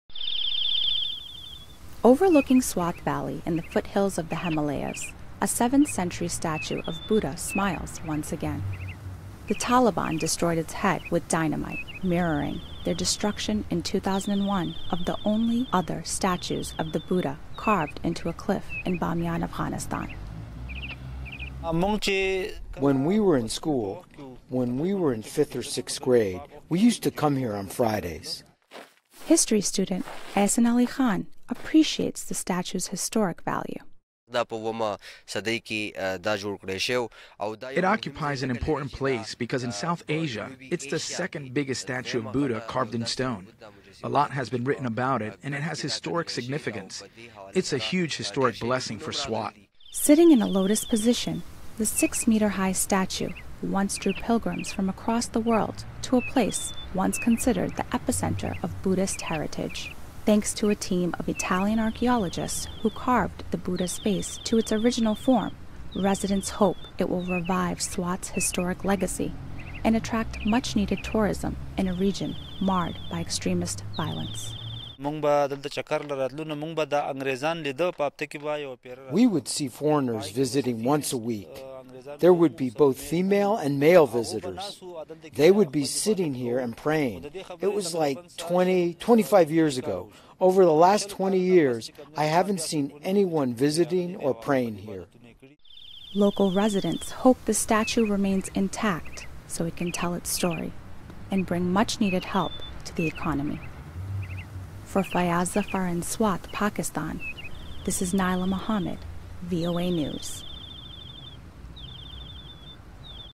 Archeologists Restore A Damaged Buddha Statue in Pakistan
Italian archeologists have recently restored a statue of the Buddha in Pakistan's Swat Valley. It is believed to be the second tallest statue in the region.